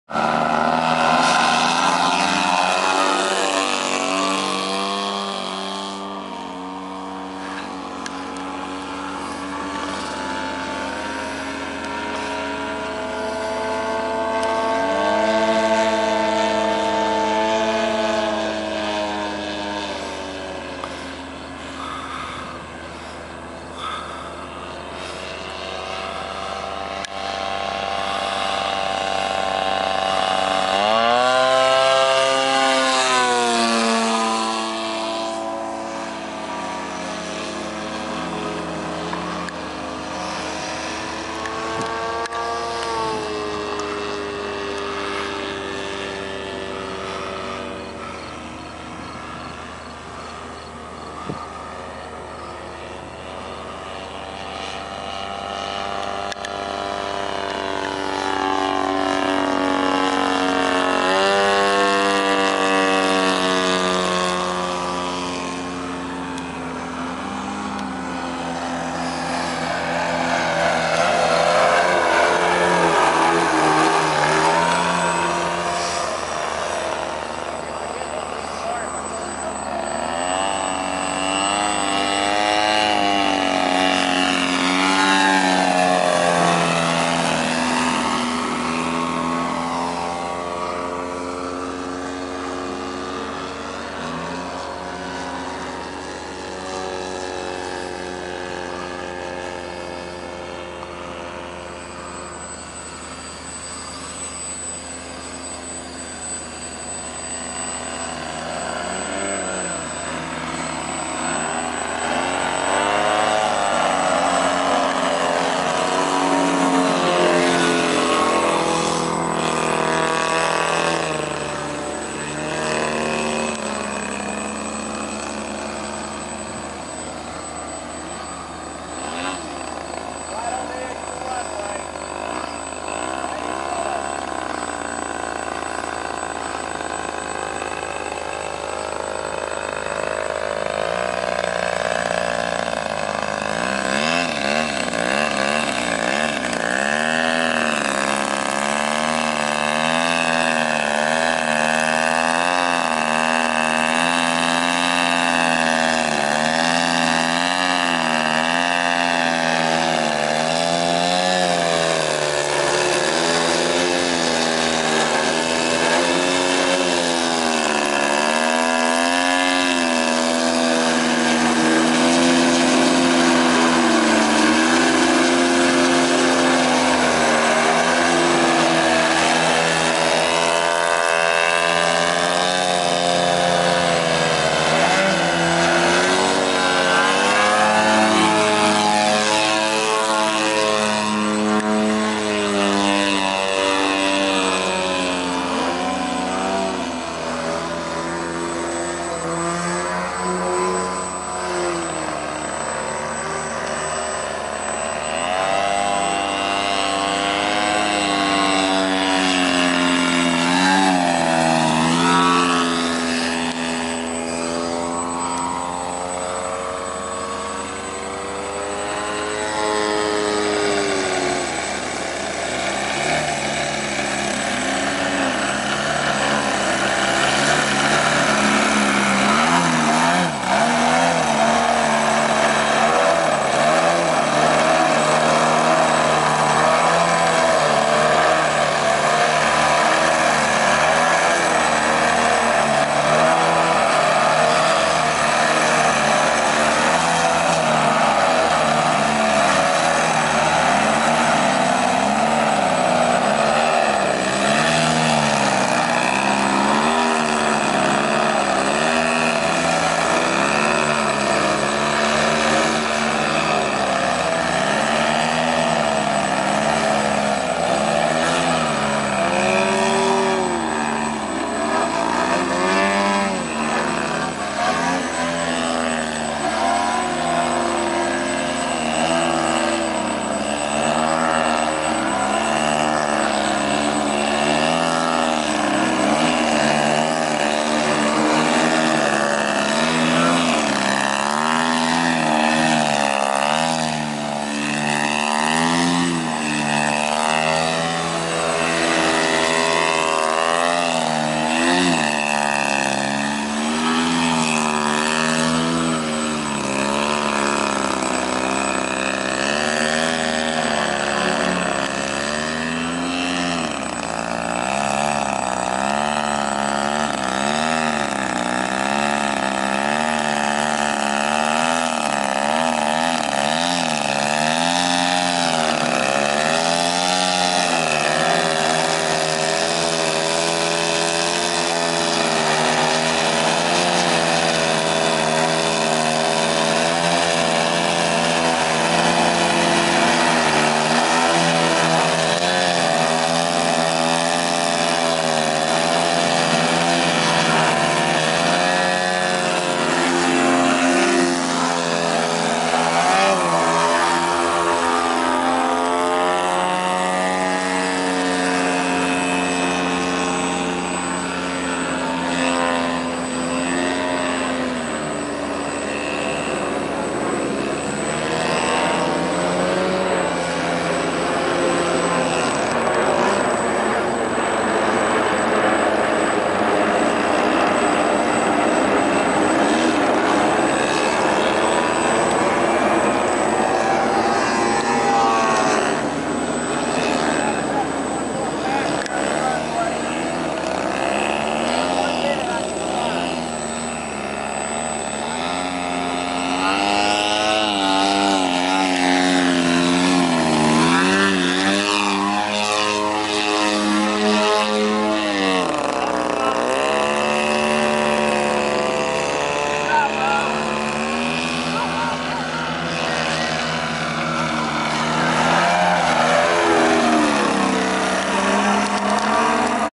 2014 Chattanooga 3D Jam - Justin and Tony Do A Little Dance
2014 Chattanooga 3D Jam Witching Hour Tandem Flight featuring Justin Owen and Tony G.